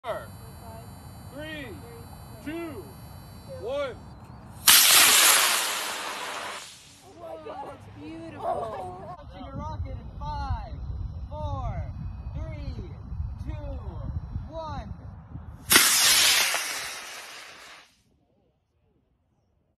NMHS Rocketry has officially started launches for the 2022-23 season! Both teams 12 & 17 had success
NMHS Rocketry has officially started launches for the 2022-23 season! Both teams 12 & 17 had successful first launches, thanks to everyone who came :).